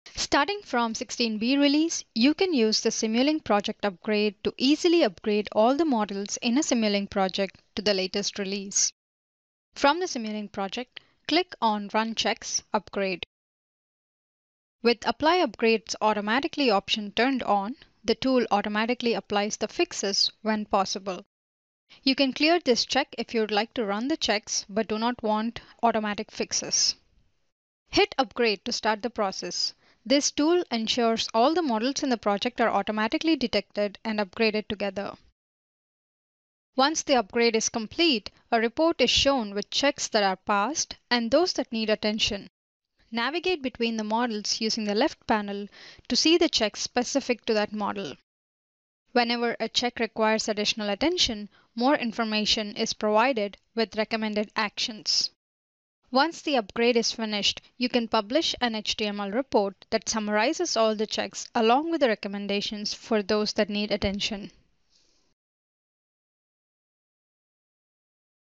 Simulink Project Upgrade
See what's new in the latest release of MATLAB and Simulink

Simulink® project upgrade lets you easily upgrade all the models in a Simulink project to the latest release. The tool ensures all the models in the project are automatically detected and upgraded together. The checks run on all models and the fixes are automatically applied.

Once the upgrade process is complete, an HTML report is shown with checks that were passed and those that need attention. Whenever a check requires additional attention, more information is provided with recommended actions.